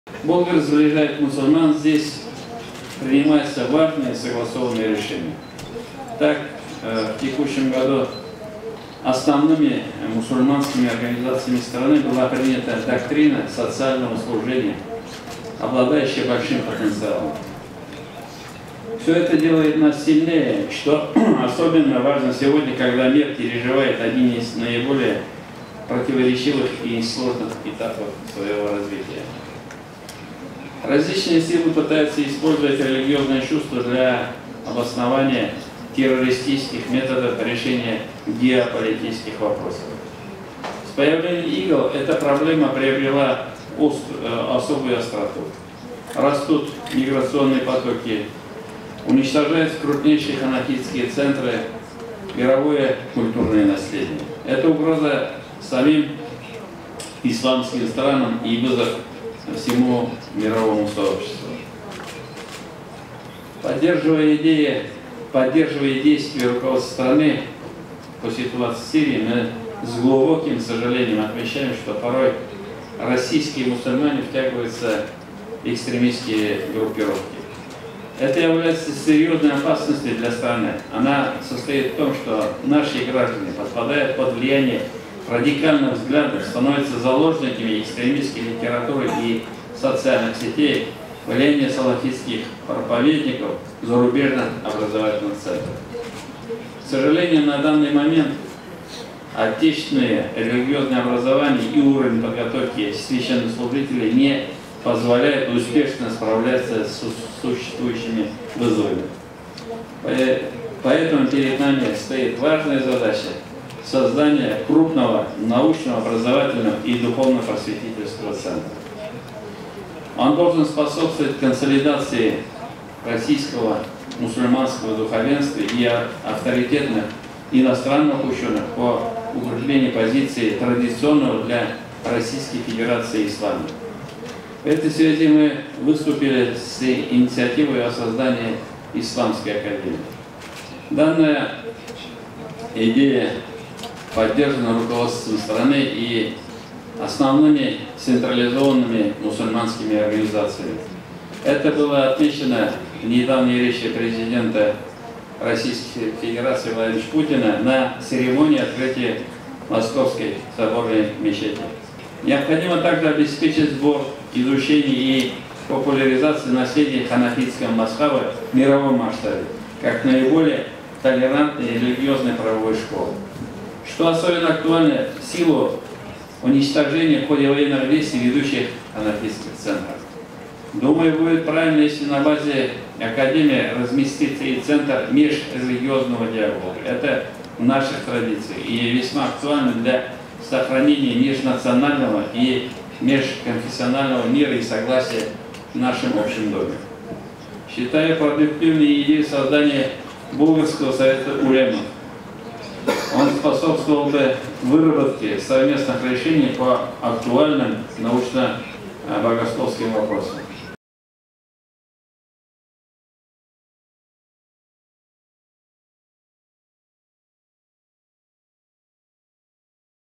Рустам Минниханов предложил открыть центр межрелигиозного диалога в Болгаре
Крупнейшая в РФ исламская академия стартовала накануне с открытия скромных полугодовых курсов по чтению Корана.